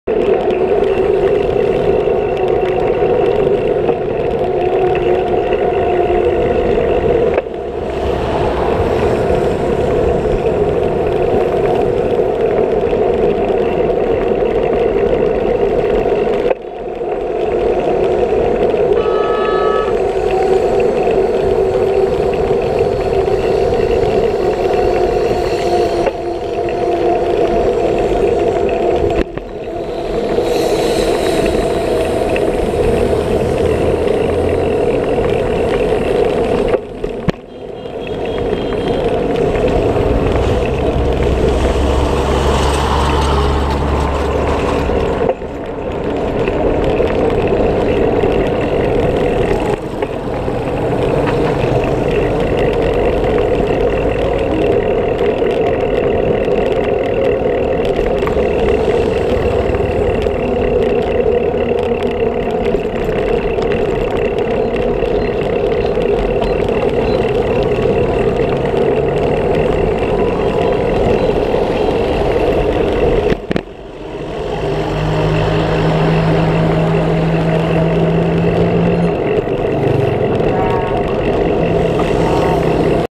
AH1/National Highway 5 on Bike. Pursat, Cambodia
Cambodia National Highway 5 on Bike. Pursat.
Stayed at Than Sour Thmei Hotel (1:22 right side, A/C, HS, TV Free-WIFI @USD15 as of Mar. 2014)